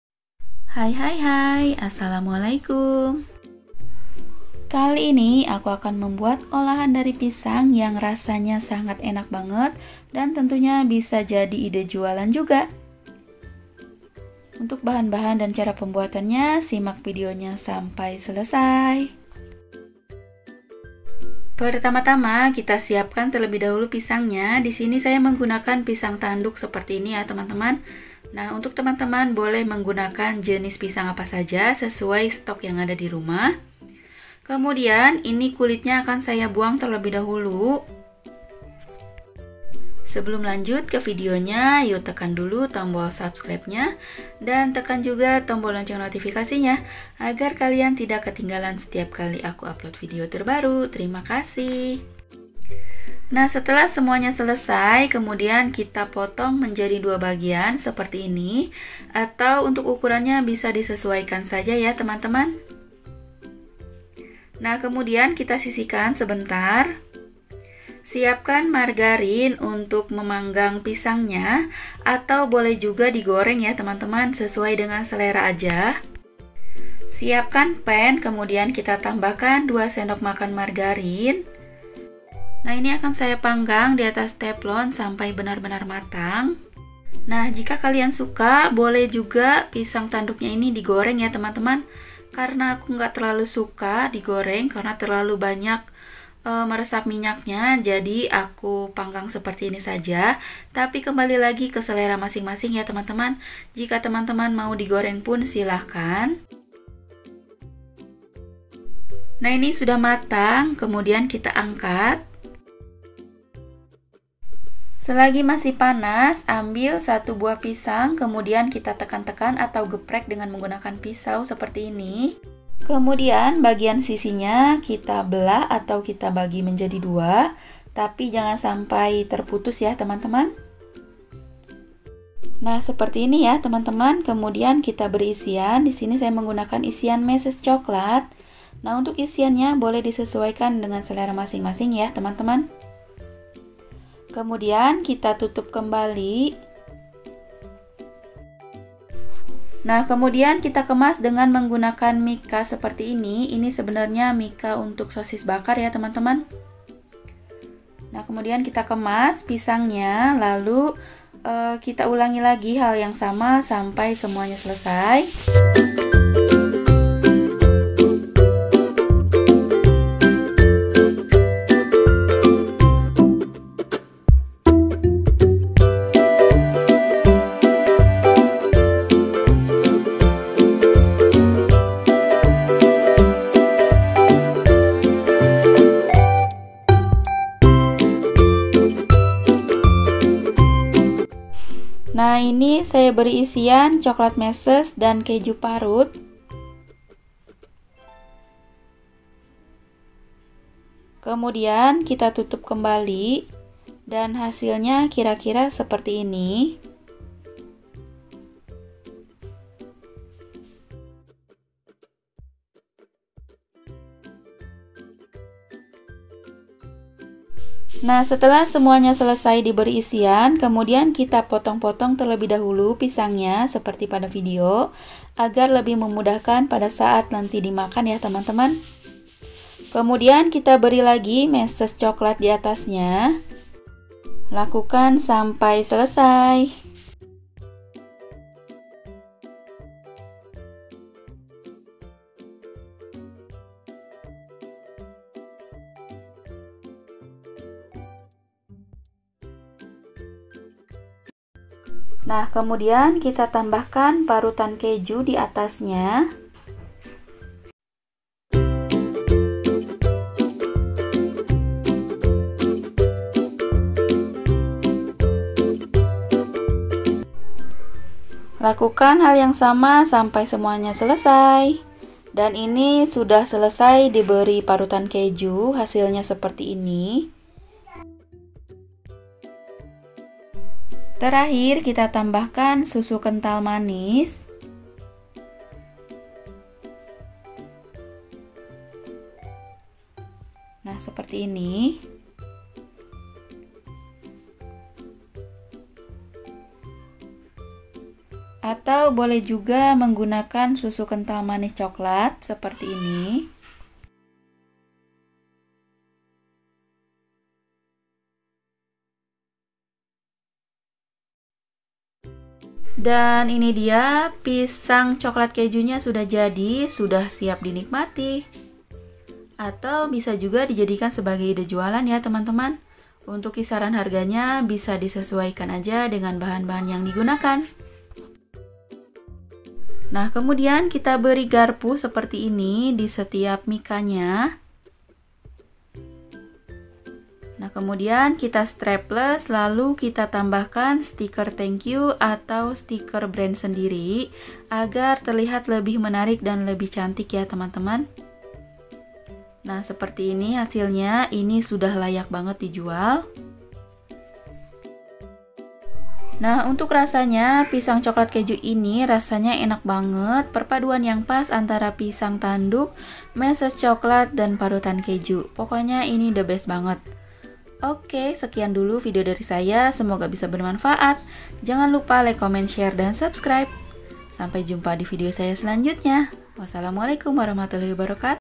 JUALAN INI MODALNYA DIKIT UNTUNGNYA BANYAK,CARA BIKINNYA MUDAH & SIMLPE BANGET❗
Assalamualaikum

PISANG TANDUK COKLAT KEJU
Bahan:
3 buah pisang tanduk
2sdm margarin / mentega
150gr meises coklat
150gr keju parut
SKM secukupnya

Untuk cara pembuatannya simak video di atas hingga selesai ya..